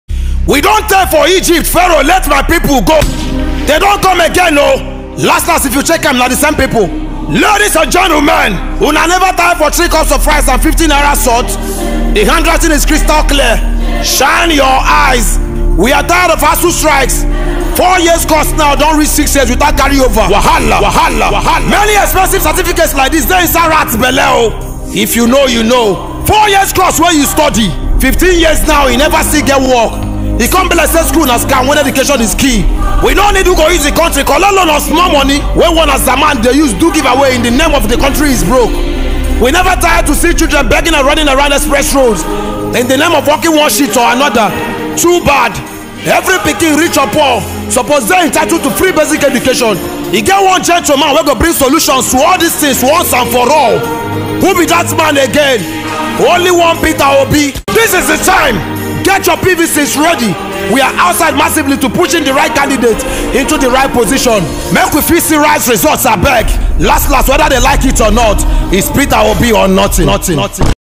PETER OBI PAINTING PORTRAIT. True leader inspires followership. I'm fully OBI-dient. .
PETER OBI PAINTING PORTRAIT
Medium: Oil Paints on Canvas.
Size: 3ft by 3ft.